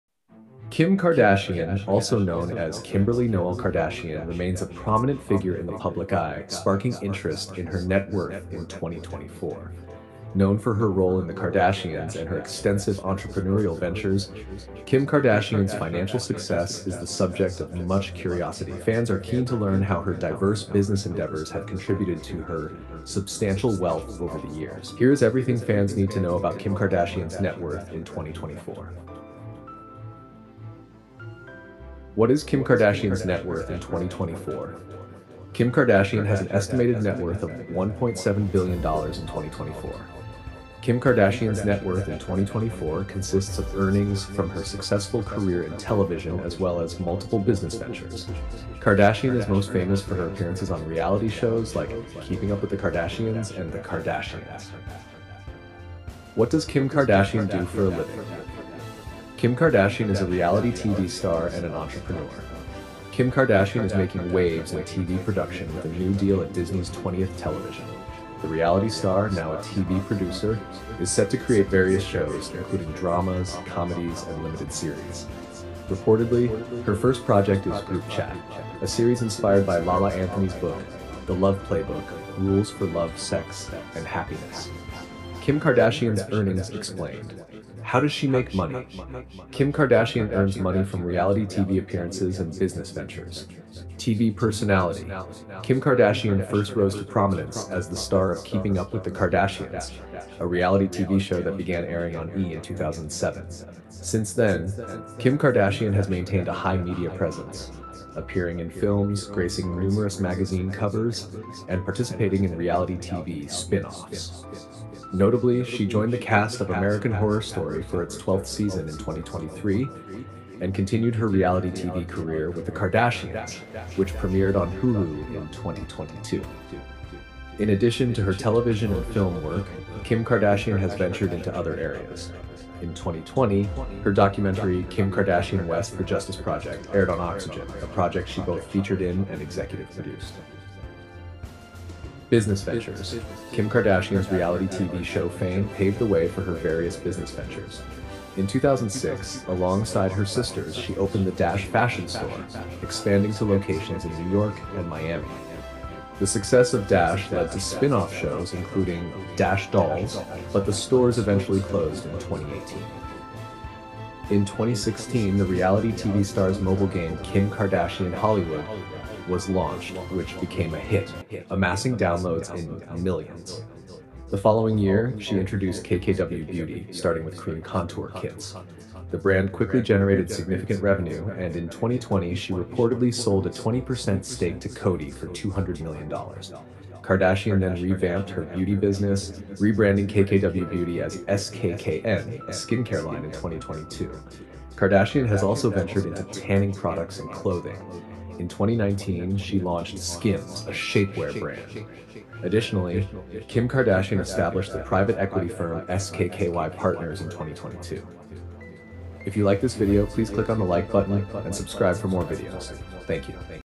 Kim Kardashian Net Worth 2024: How Much Money Does She Make?
Kim Kardashian or Kimberly Noel Kardashian remains a prominent figure in the public eye, sparking interest in her net worth in 2024. Known for her role in The Kardashians and her extensive entrepreneurial ventures, Kim Kardashian’s financial success is the subject of much curiosity. Fans are keen to learn how her diverse business endeavors have contributed to her substantial wealth over the years.

Here is everything fans need to know about Kim Kardashian’s net worth in 2024.